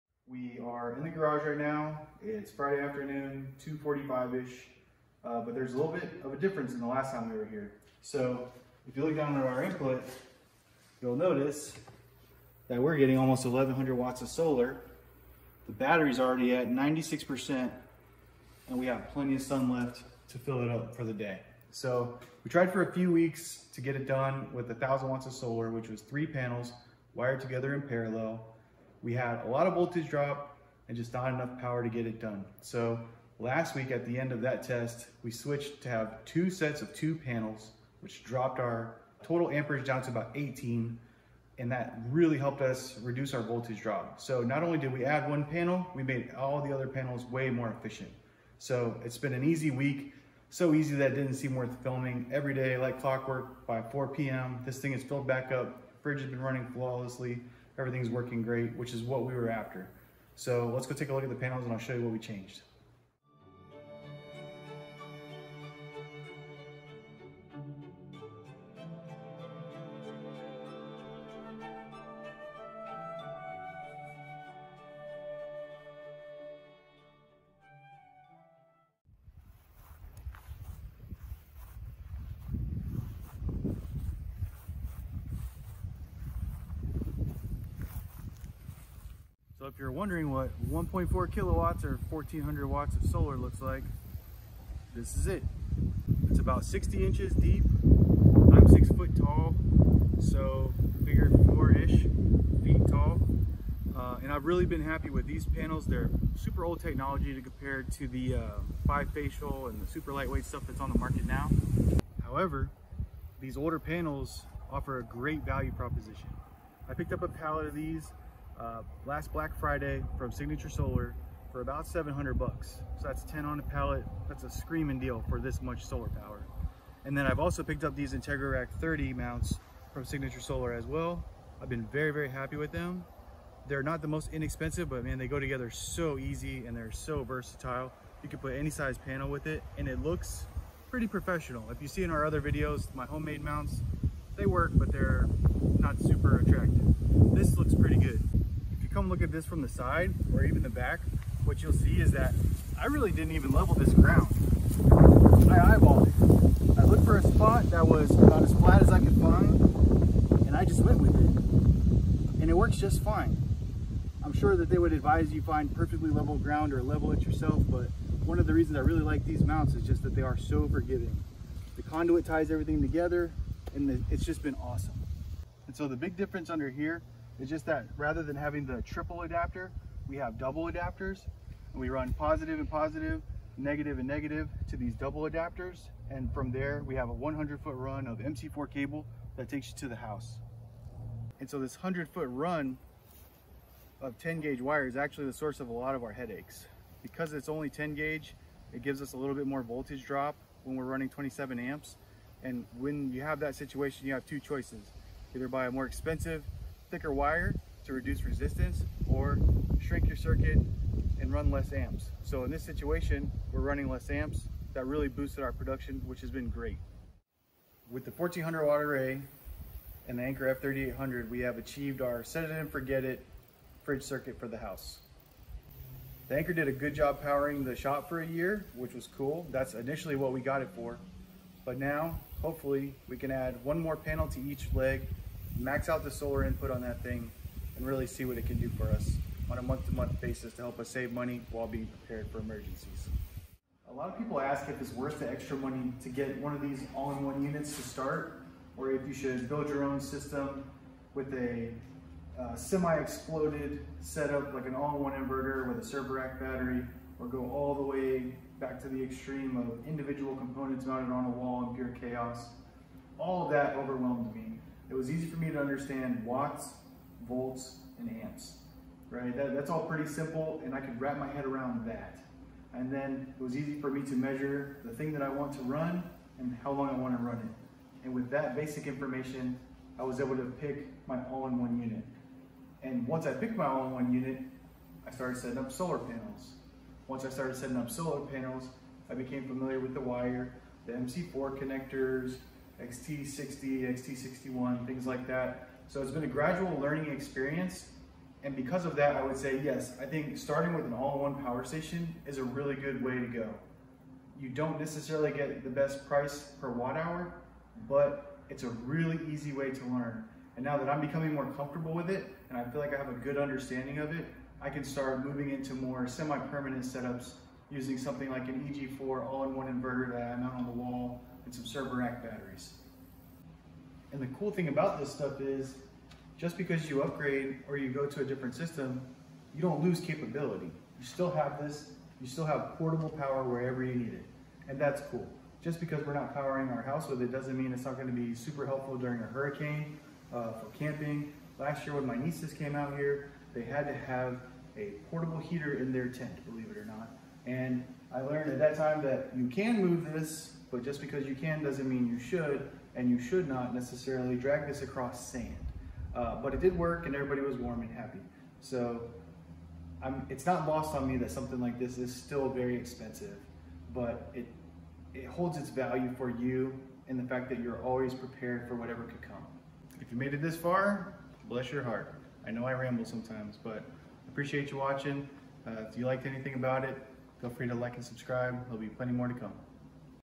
Off-Grid Power Test: 1000 Watts Failed - Here's What Finally Worked
Welcome back to Backyard Florida! 🌴
Today, we’re ending our first off-grid power test and we definitely learned a lot about real power consumption and what we actually need to get even partially off-grid on the homestead⚡

This is the conclusion to this test, but it is only the beginning of our solar journey so if you’re into off-grid living, solar power, and backyard experiments, you’re in the right place!

🌞 Solar power tests
🔌 Off-grid living projects
🏡 Florida backyard experiments

00:00 Introduction
01:29 Changes We Made
03:37 Test Goal Achieved
04:04 All-in-one unit or build your own system?
06:10 Conclusion